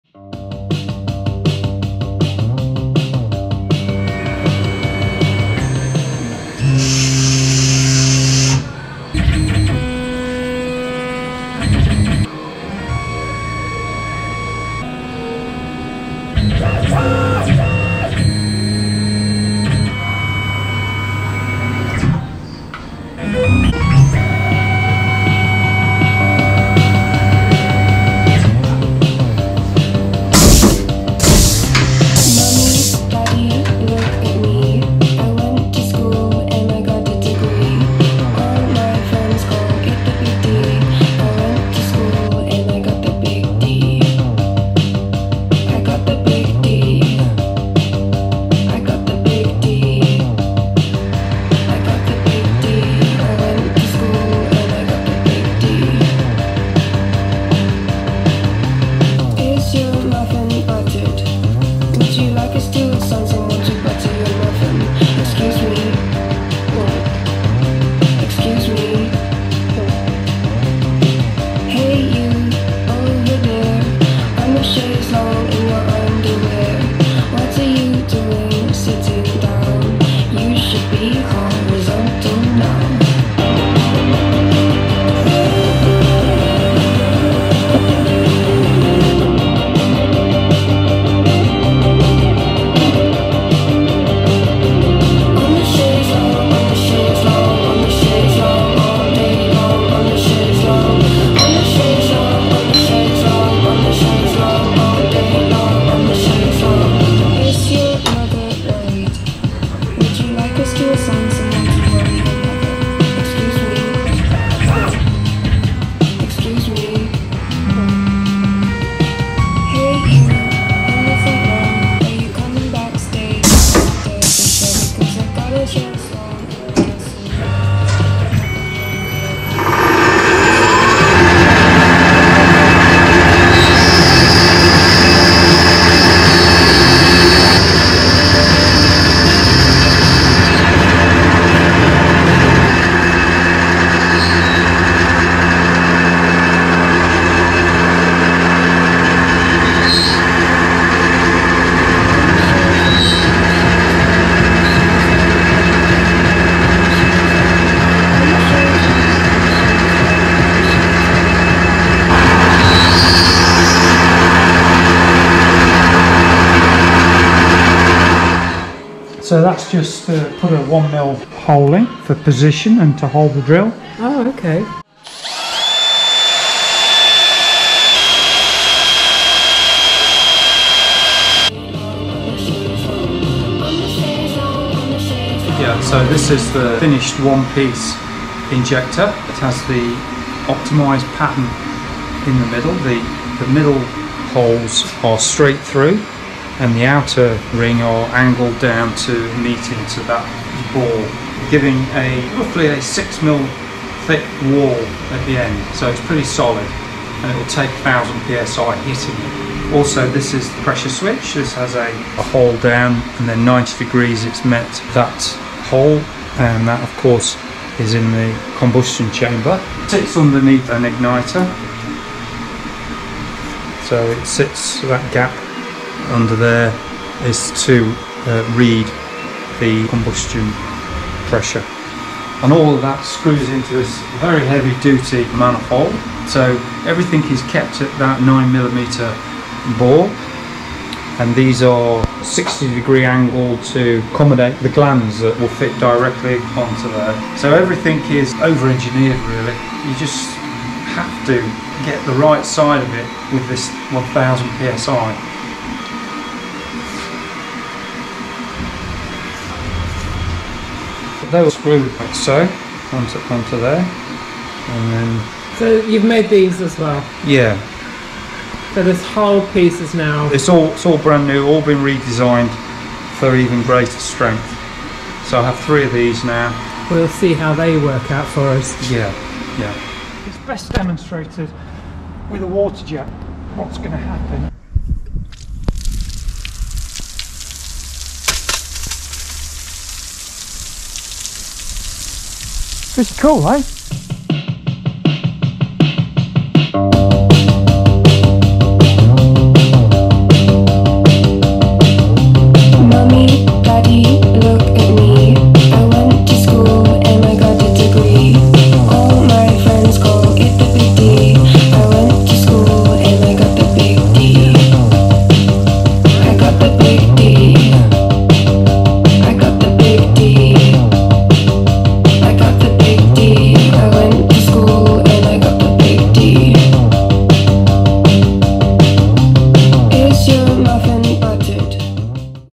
CNC Milling Work on Rocket One Piece Injector Block | Tormach CNC | Hybrid Rocket Engine Production
Here we have Michael finishing of the work on the New Optimised one piece injector block.
Previously this has been an issue, on the last launch the injector block was an after market attachment and flew into the combustion chamber under pressure of 1000psi, and didn’t have the objective of chocking.
Now he has designed and manufactured the whole of the injector block, strengthening where possible, and has incorporated a whole new gland block too.  Hopefully this new piece will be man enough to take the pressure.
The next launch is not to far away now